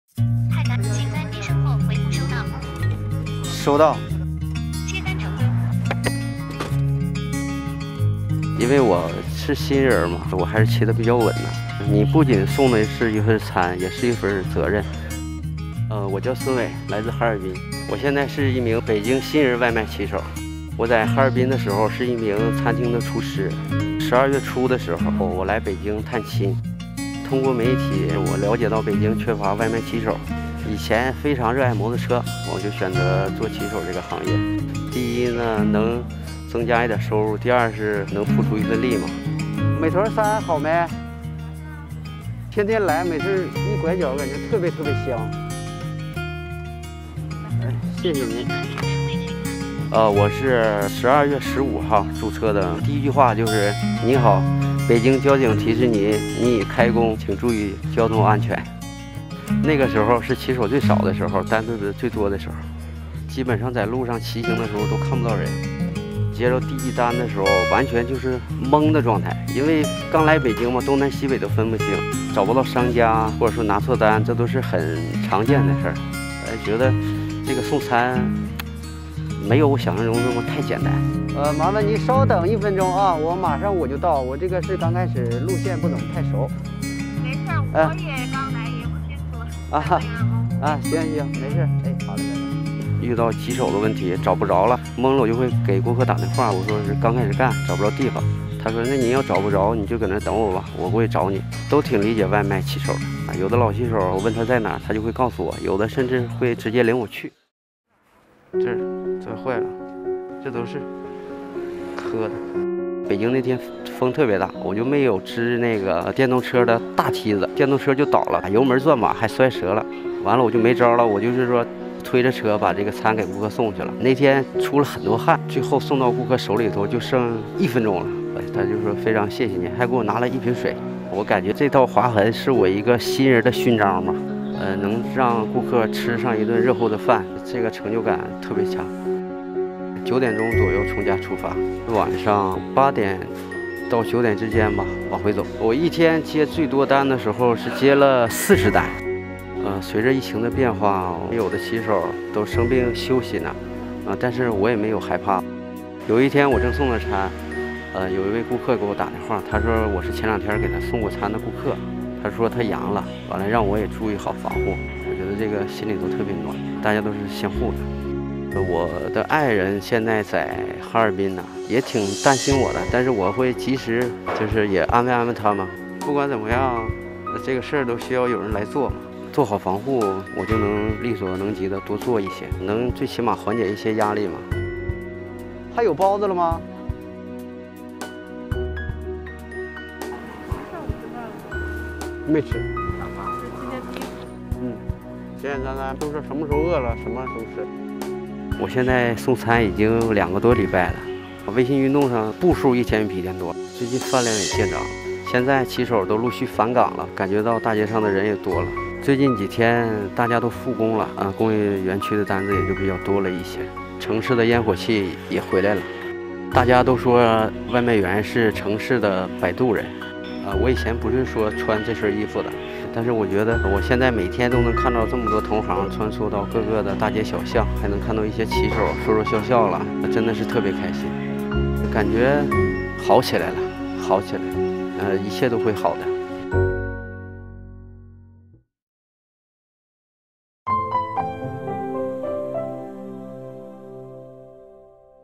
北京新人外卖骑手：每天12小时在路上 想做些力所能及的事
来自哈尔滨的孙伟，12月初来到北京探亲。那时受疫情影响，北京外卖订单量骤增。在网上看到号召居民群众加入骑手行列的倡议后，孙伟心一横，就这样留了下来，成为了一名新人外卖骑手。拿错过单、迷过路……在顾客的理解和老骑手的帮助下，孙伟一点点克服对于新人骑手的考验。现在的他每天有12个小时骑行在路上，微信步数一天比一天多，饭量也见长。他总说，做好防护，想力所能及多做一些。